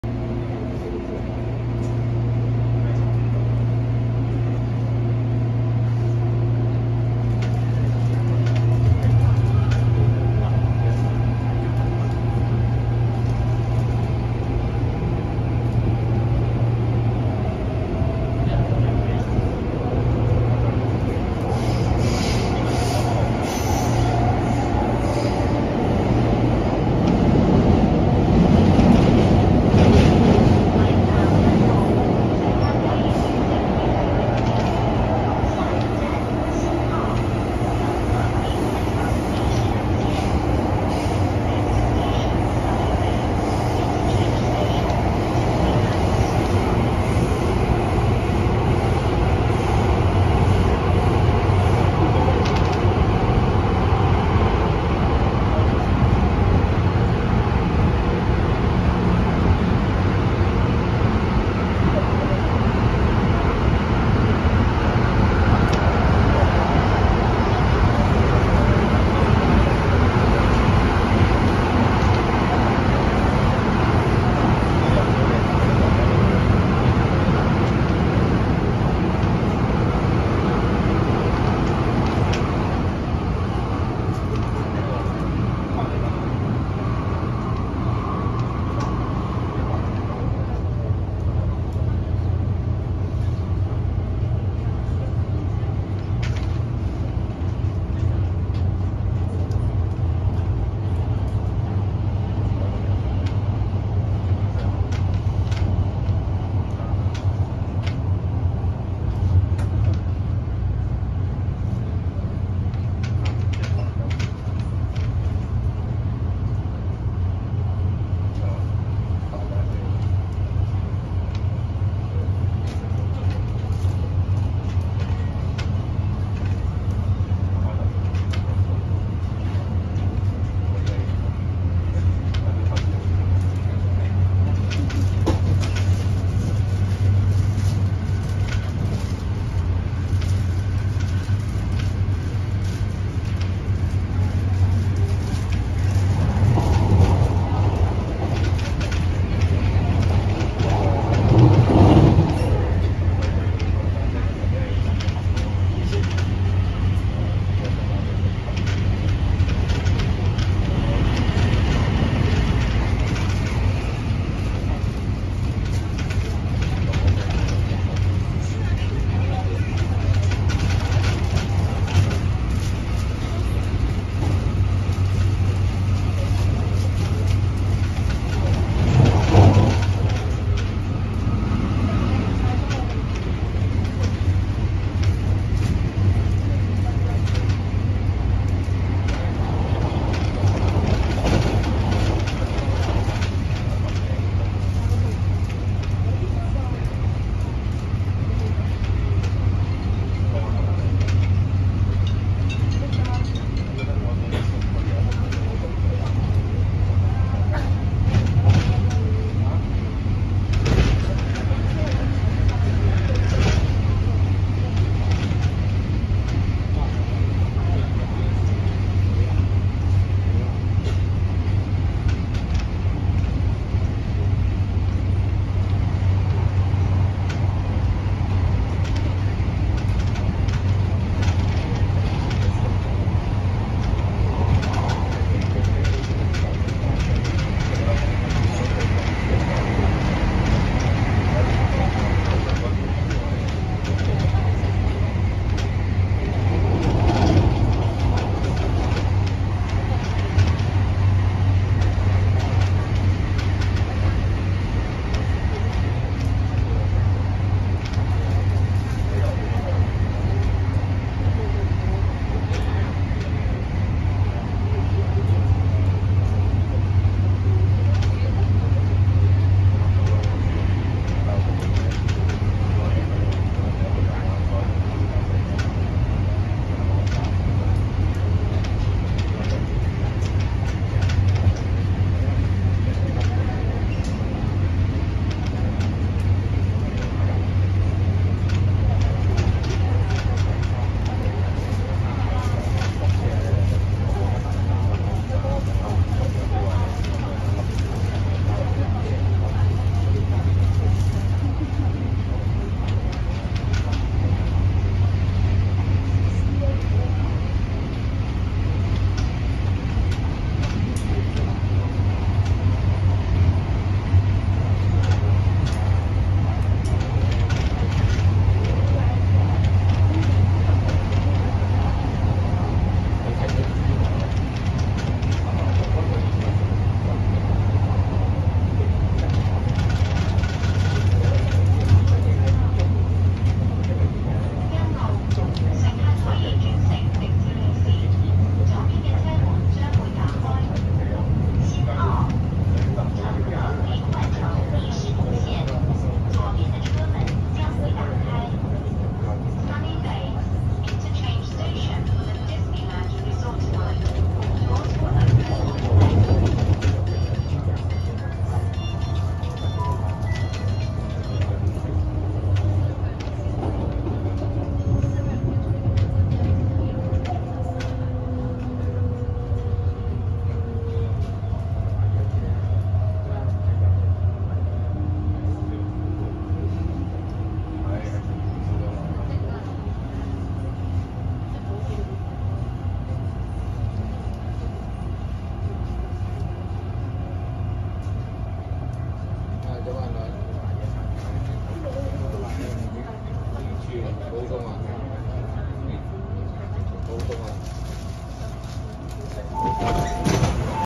MTR CAF-Stock EMU (V606/V806) Adtranz-CAF, Tung Chung➡️Sunny Bay (Tung Chung Line)
港鐵機場鐵路Adtranz-CAF列車（英語：Lantau Airport Railway Adtranz-CAF Train，簡稱LAR Train或CAF-Train）是港鐵公司（前稱地鐵公司）為其東涌綫及機場快綫而購入的列車。該列車由西班牙的CAF，以及德國與瑞士的Adtranz（2001年由加拿大龐巴迪公司收購，現為阿爾斯通旗下）製造，機場快綫全線11列及東涌綫12列列車均用此電聯車。此乃香港歷史上第一款非由英國廠商製造的通勤型電聯車。

The Adtranz-CAF EMU (also known as CAF-Train or LAR-Train) is an Electric Multiple Unit (EMU) train that operates on the MTR rapid transit railway system in Hong Kong. There are two variants, one used on the conventional Tung Chung line, and a more luxurious version used on the Airport Express. The vehicle interiors were designed for MTR by Jones Garrard, while the vehicle exterior and driver's cab interior were designed in the UK by Design Triangle (the first two modifications of these were designed in Spain by Diara Design ). These two variations are built jointly by Adtranz (acquired by Bombardier Transportation in 2001) and Construcciones y Auxiliar de Ferrocarriles (CAF) and manufactured in Spain in 1996-7.